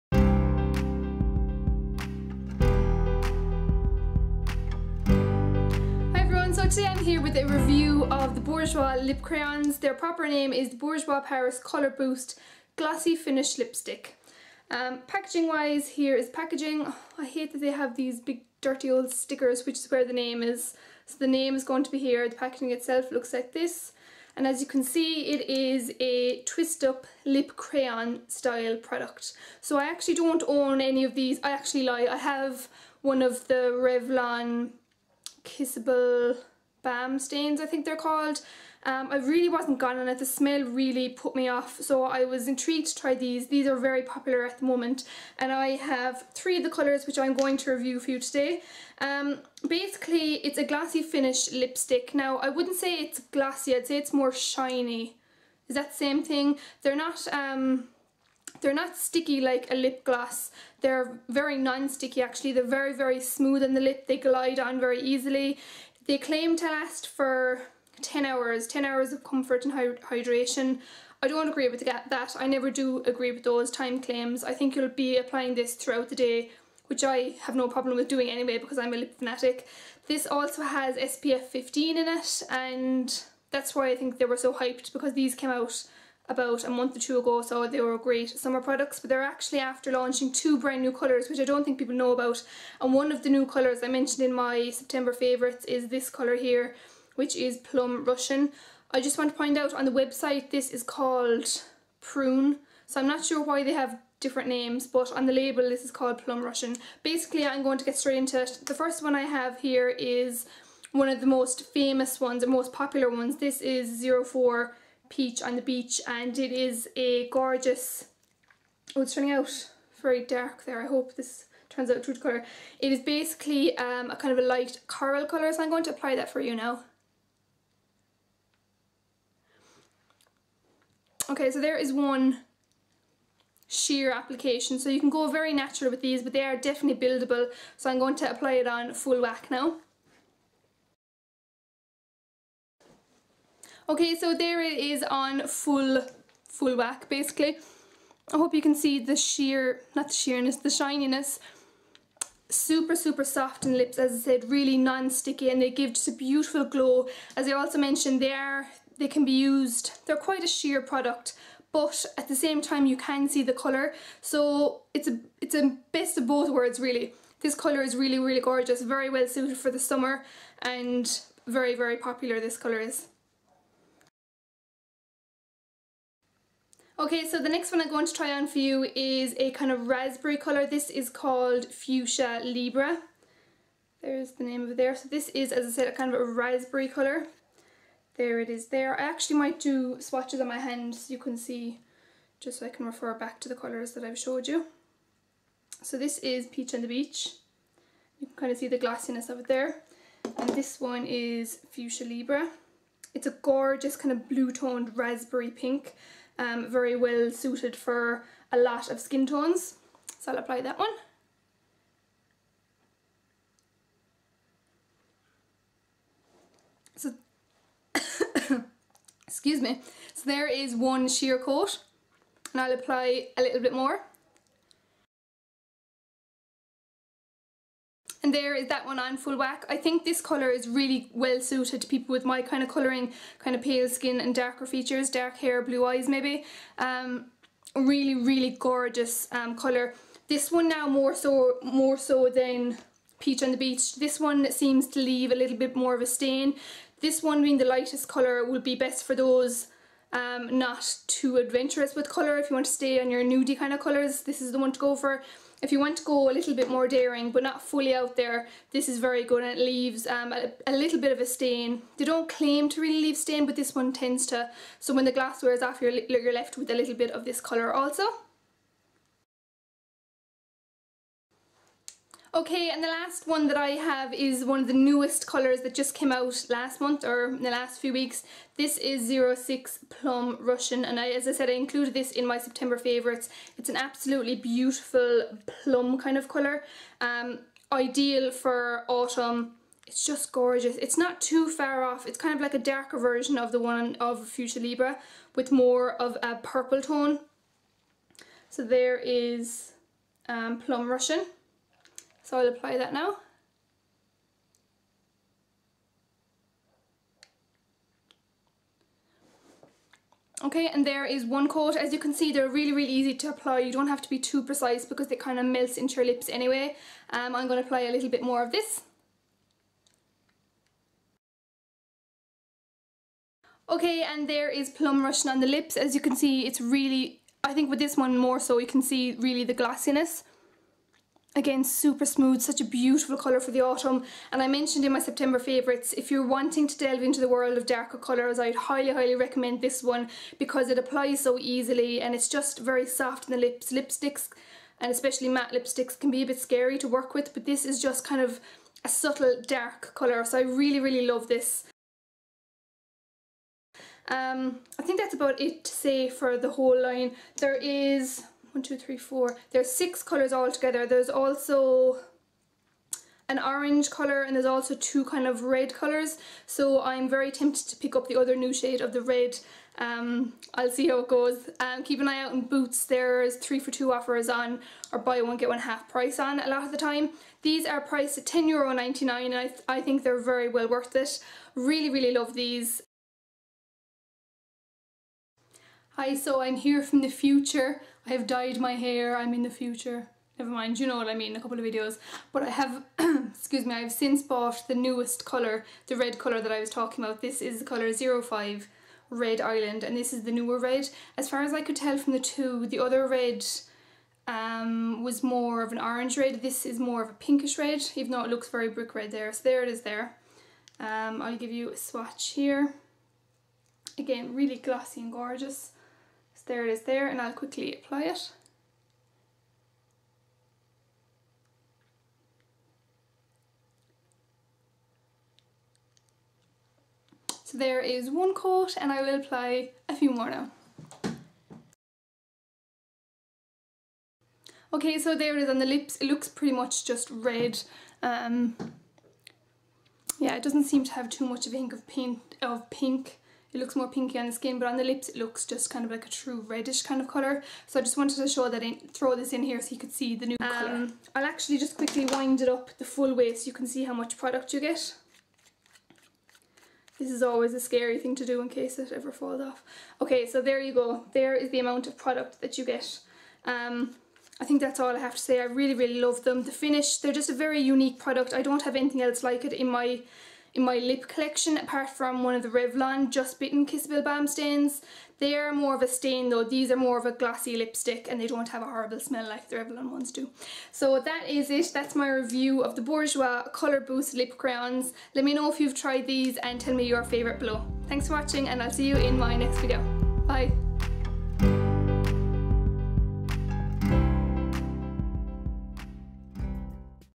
Bourjois Color Boost Glossy Finish Lipstick Review
Available in 6 Shades:
01 Red sunrise
02 Fuschia Libre
03 Orange punch
04 Peach on the beach
05 Red Island
06 Plum Russian